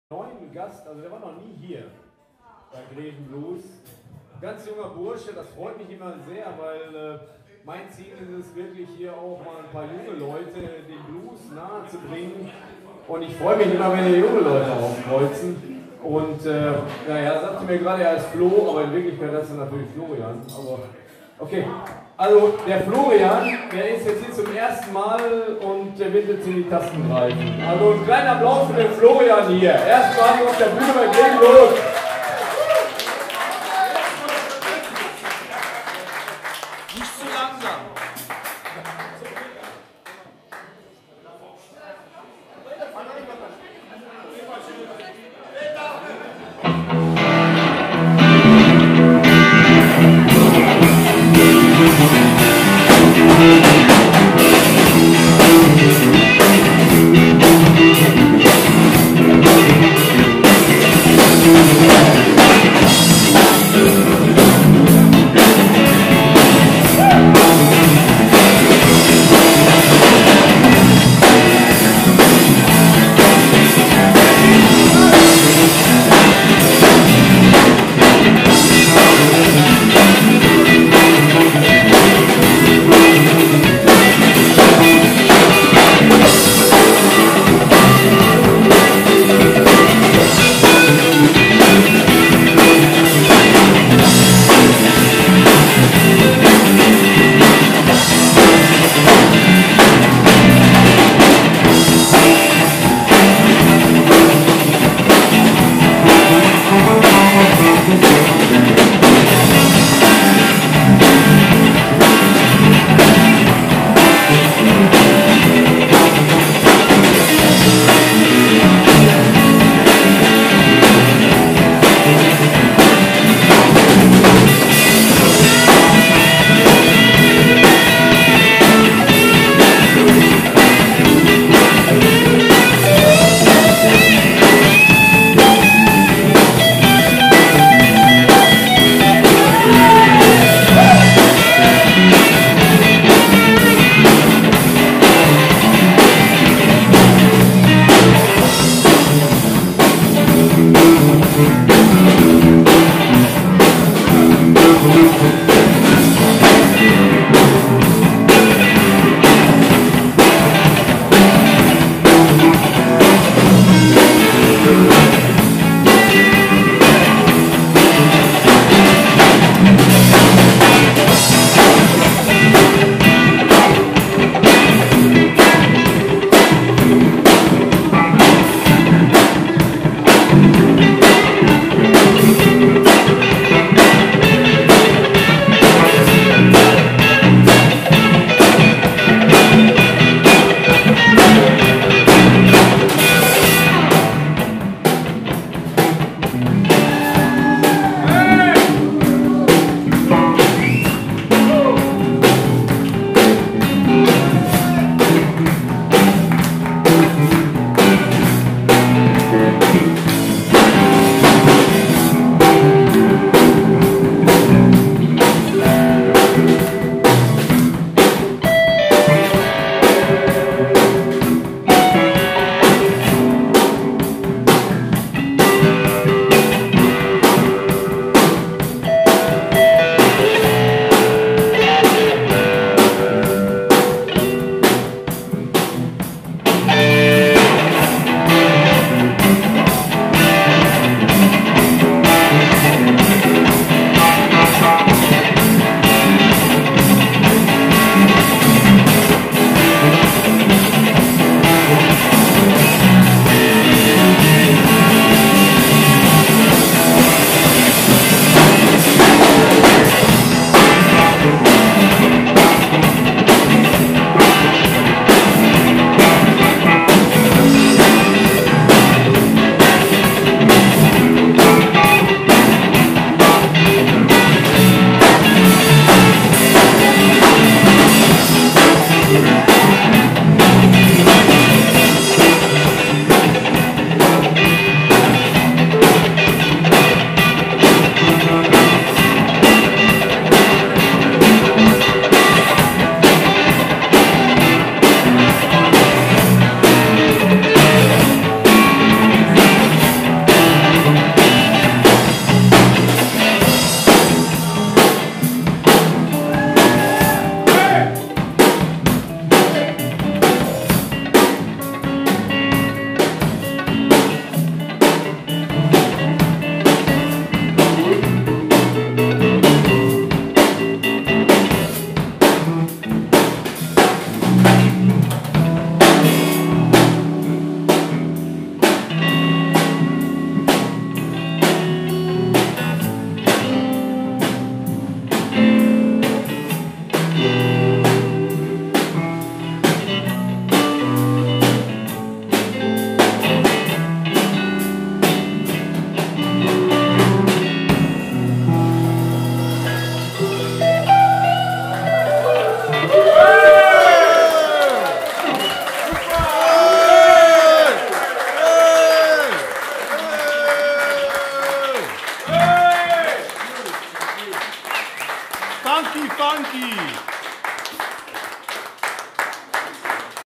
GrevenBLUES - Der Nachwuchs - Grevenbroich - Kultus - 20.04.2018 (4/8)
Bluessession ( Der Nachwuchs)
Gitarre - Florian
Bass - Marion Tight
Schlagzeug - Uli Löhrwald